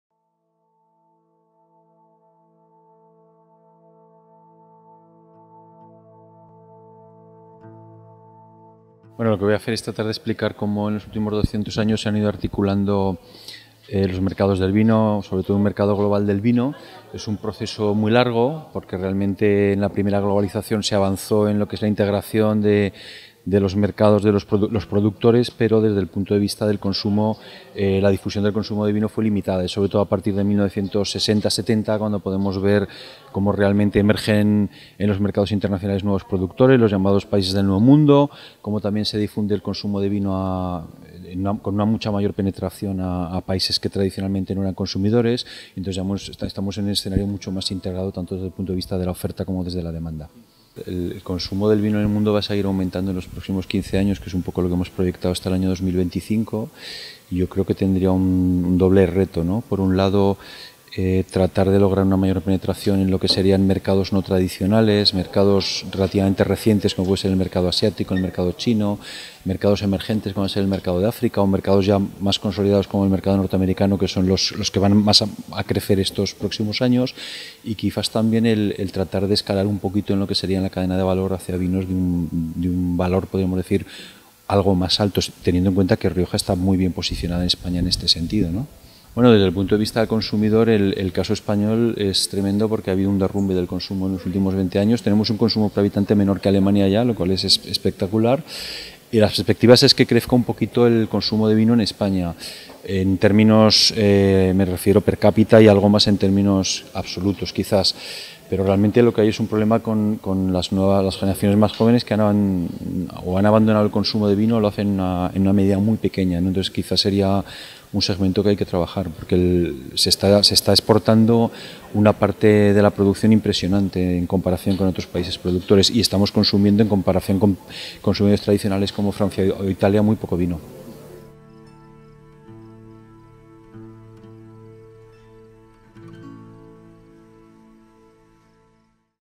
'Doscientos años de globalización del vino', Vicente Pinilla Navarro. UCC+i, Universidad de La Rioja
La conferencia 'Doscientos años de globalización del vino: una nueva historia comparativa', a cargo de Vicente Pinilla Navarro, ha dado comienzo a las actividades del Aula Pedro Vivanco Cultura del Vino de la Universidad de La Rioja.